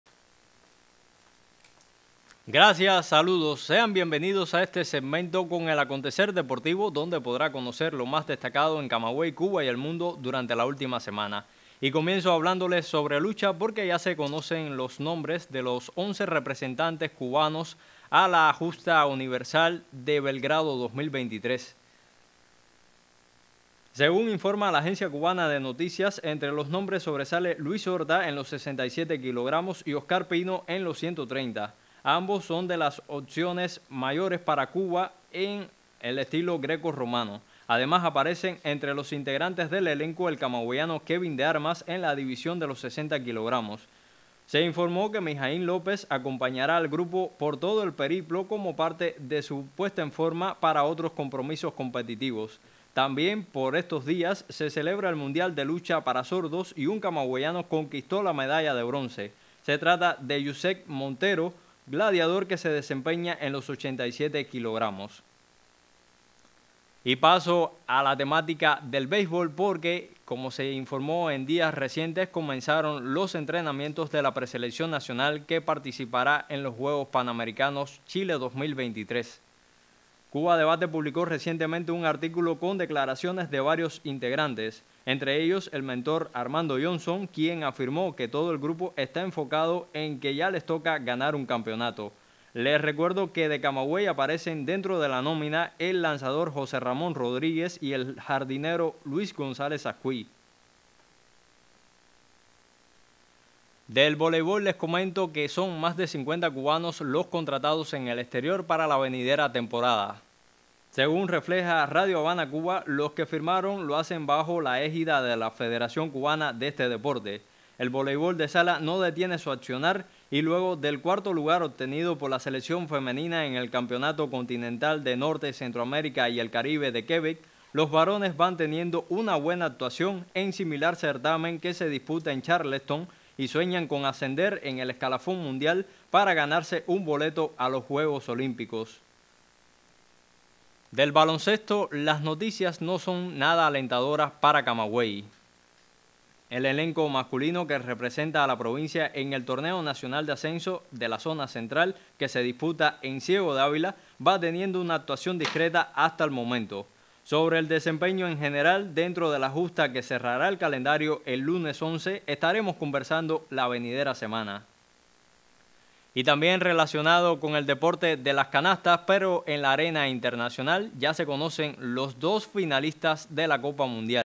Estados Unidos queda en el camino en Mundial de Baloncesto
En Acontecer Deportivo cuento lo más destacado de Camagüey, Cuba y el mundo durante la última semana.

Lo más destacado:
- Cubanos al Mundial de Lucha 00:00
-  Preselección de béisbol 01:05
- Voleibolistas contratados en el exterior 01:43
-  Camagüey en el TNA masculino 02:19
- Serbia vs Alemania en la final del mundial 02:45

Cámara: Ángel García
Audio: Linardo Méndez
Luces: Seglhan Lombida
Montaje: Oscar Otaño